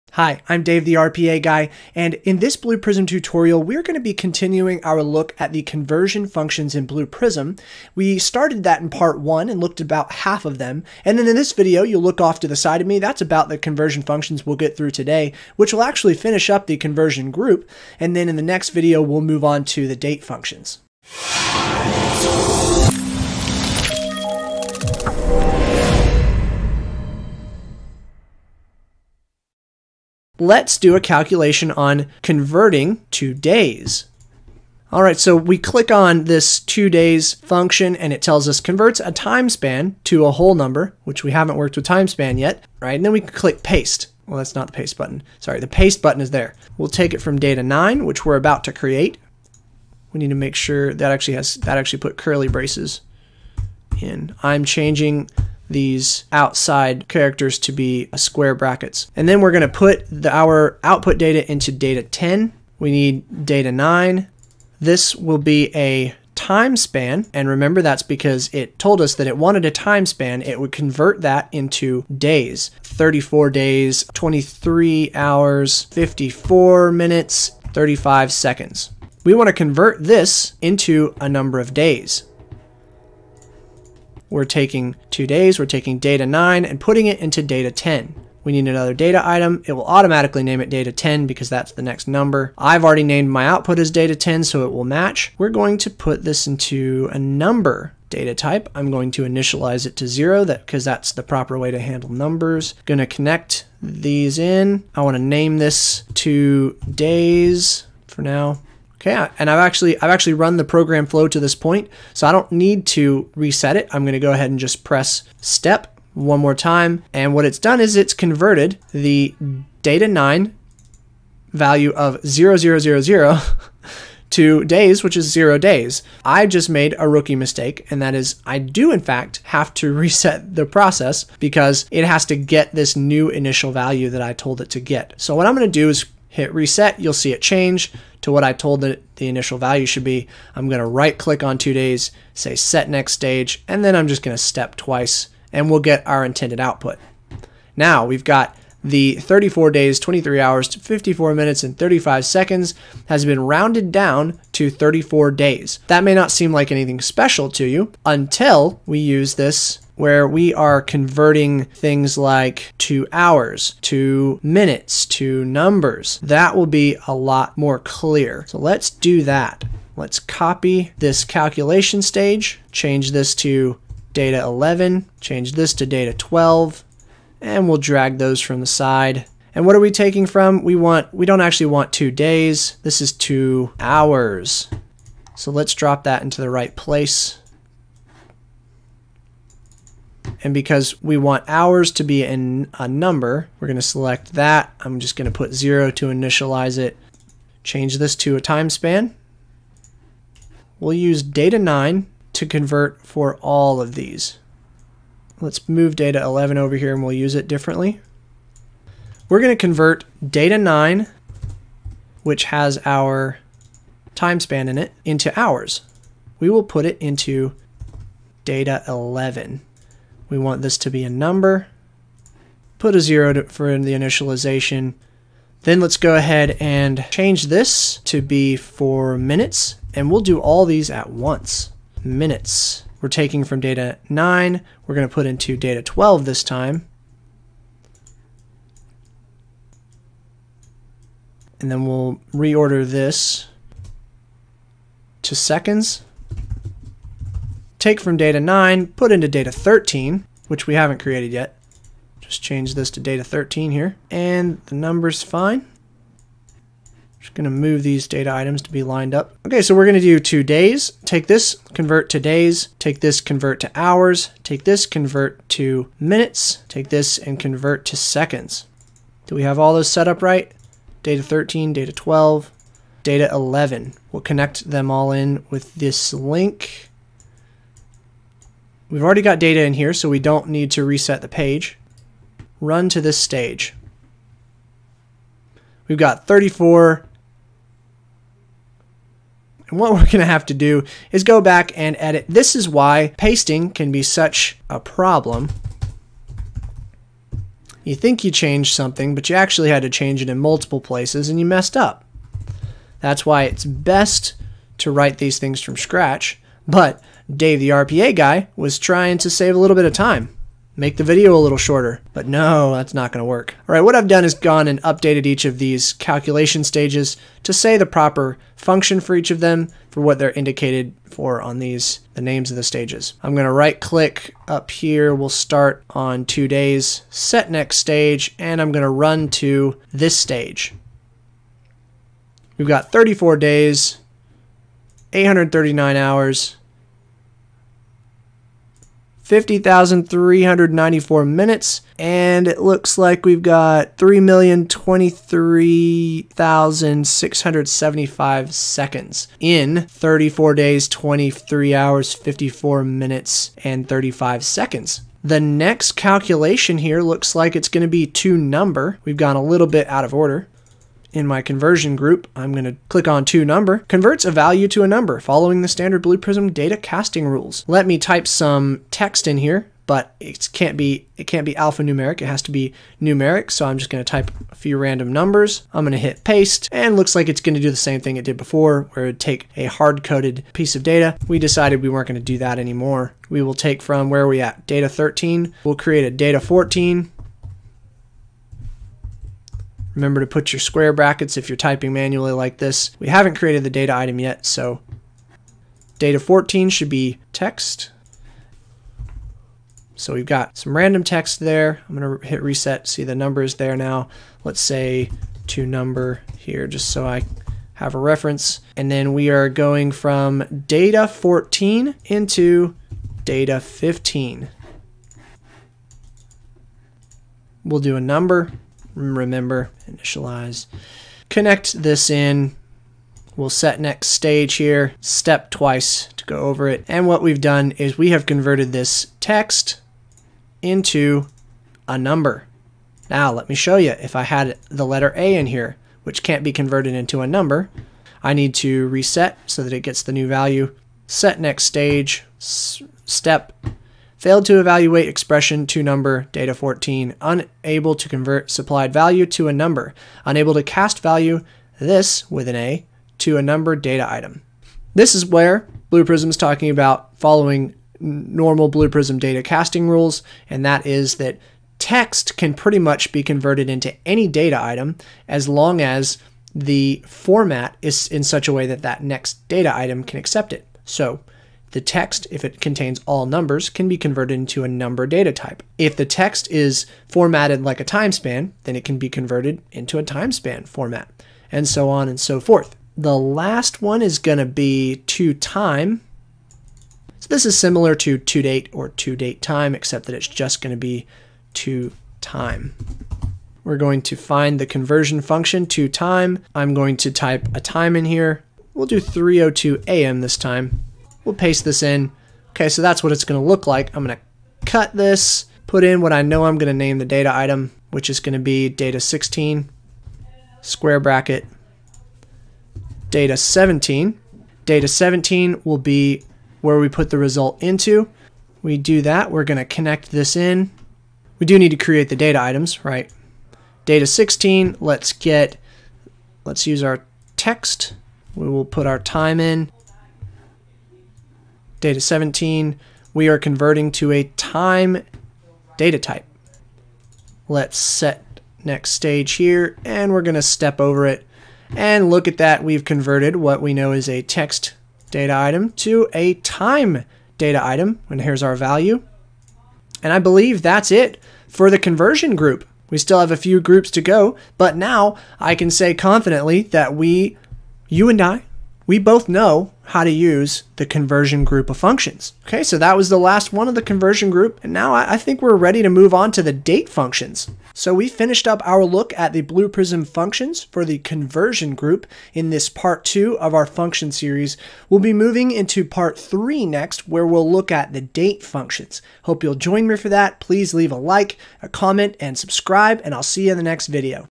Blue Prism | Tutorial | Functions | Part 2 - Conversion continued
Blue Prism Tutorial showing how to use six of the Conversion Functions: ToDays(), ToHours(), ToMinutes(), ToNumber(), ToSeconds(), and ToTime().

This video is part 2 of the series for Blue Prism in-built functions. I show in this video how to give the proper inputs to these functions so that the data can be converted from one data type to another or from one data format into another.
Versions:
Blue Prism 6.2.1
Windows 10
SQL Server Express 2017